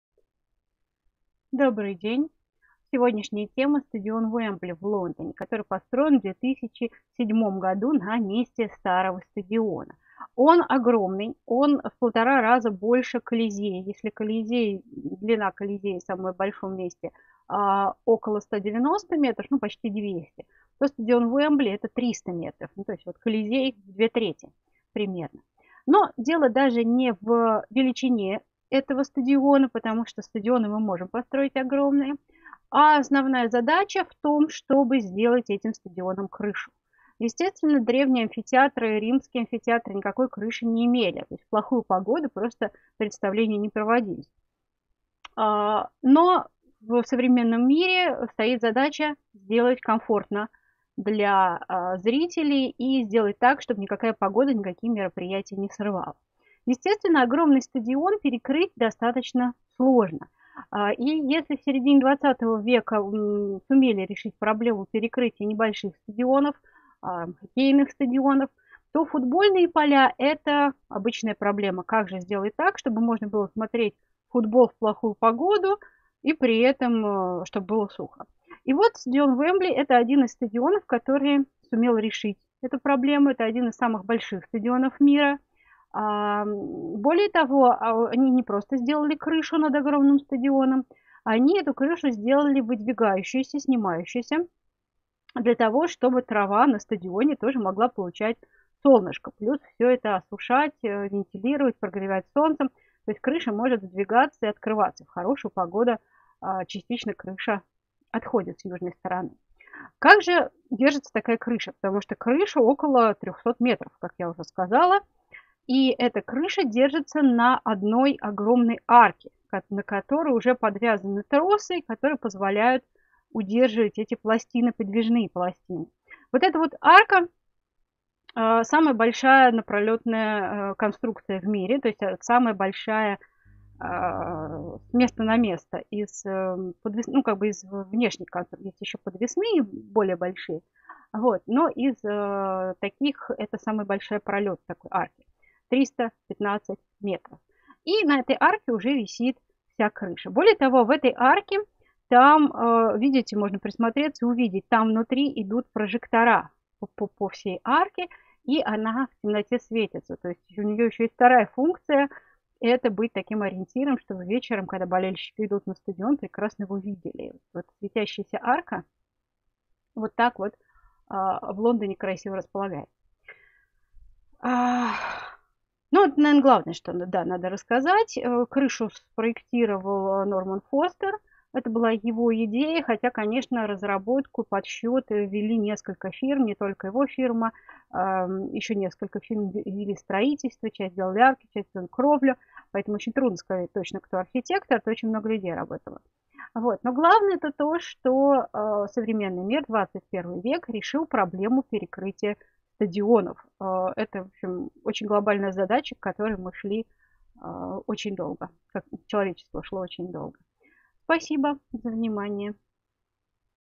Стадион Уэмбли
Стадион Уэмбли – один из самых больших, интересных и знаменитых перекрытых стадионов. Колизей в принципе не мог быть перекрыт. А теперь мы можем перекрывать такие стадионы.